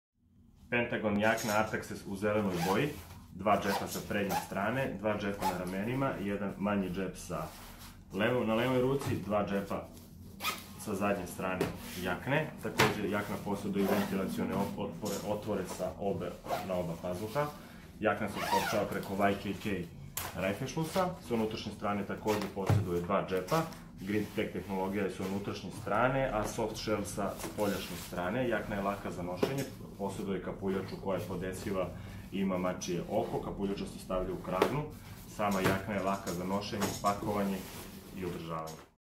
Pentagon Jakna Artaxes Zelena
OPIS:
- Napravaljena od Soft-shell tkanine koja je prijatna, mekana, udobna i elastična.
- Ima ventilacioni sistem ispod pazuha i na leđima.
- Grid-Tech postava je obrađena najfinijom vunenom tehnologijom.
- Dvosmerni YKK sistem zakopčavanja. Može da se otkopčava tj. zakopčava i od stomaka i od glave.
- Poseduje čičak panel na rukavima za kačenje amblema, grbova i drugih obeležja.
- Čičak panel je visine 13 cm x širine 8 cm.
- Vodootporna kapuljača se može lako izvaditi i vratiti u kragnu po potrebi.
- Modeli su dostupni u raznim bojama.
- Ima 9 džepova.
- Storm|Tex pruža 8000mm vodootpornu i 3000mm prozračnu (ventilacionu) membranu.
- Kapuljača sa svoje spoljašnje strane poseduje mačije oko, koje pomaže u raspoznavanju, signalizaciji itd.